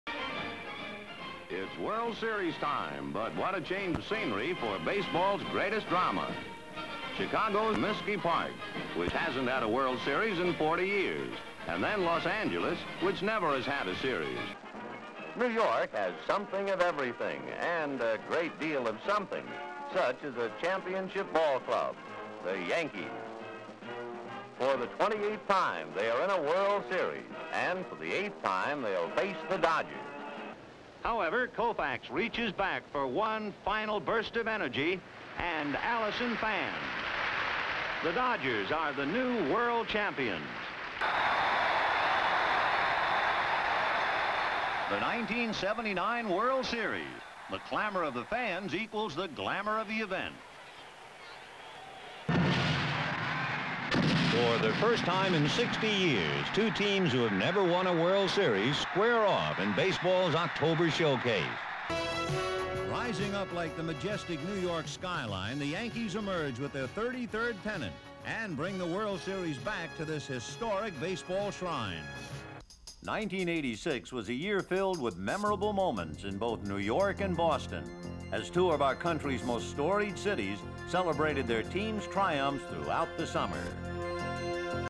Vin Scully: the voice of baseball! Listen to him narrate the openings of some classic World Series'!
Vin Scully had one of the most illustrious careers as a broadcaster. He was the voice of baseball and the World Series, narrating countless World Series openings and documentaries. Check out some of his best here!